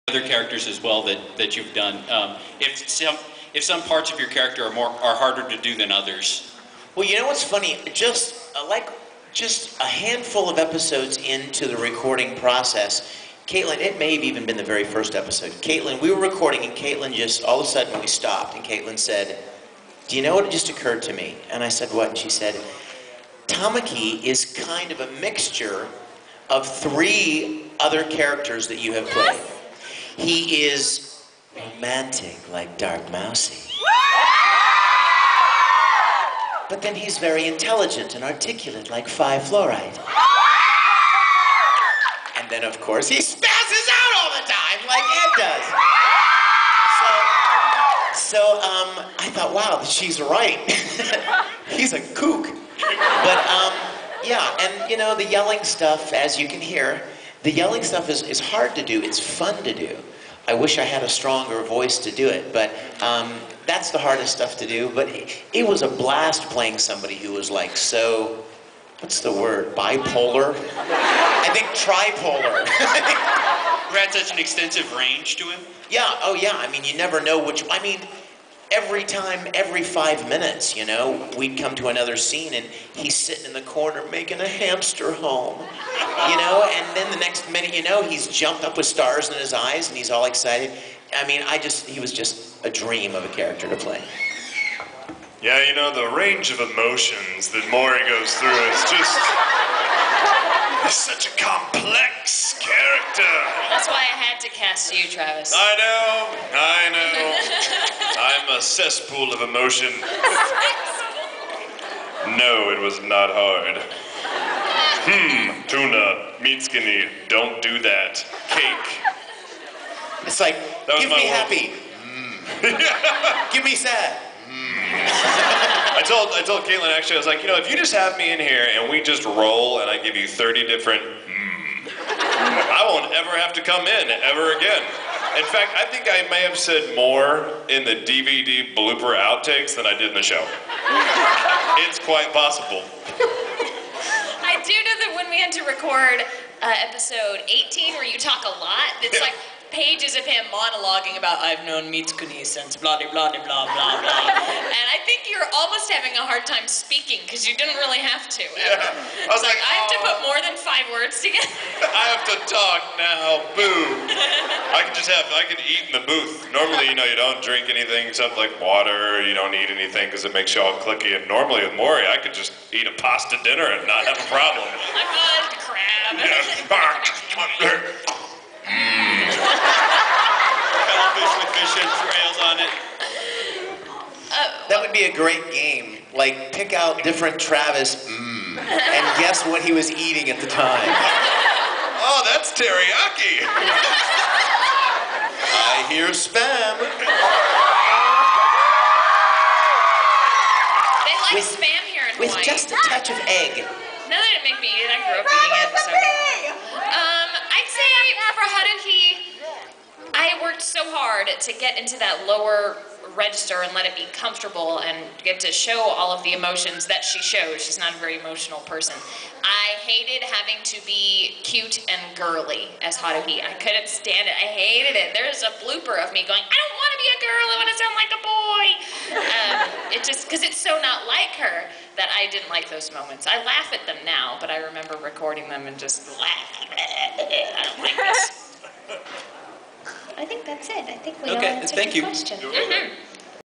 Kawaii Kon 2009 - Ouran Panel - Hard Parts of a Character
Vic Mignogna, Travis Willingham, and Caitlin Glass discuss the hard parts of playing the voices of Tamaki Suoh, Takashi Morinozuka, and Haruhi Fujioka at the Ouran High School Host Club Panel. Kawaii Kon 2009. Hawaii Convention Center. April 11, 2009.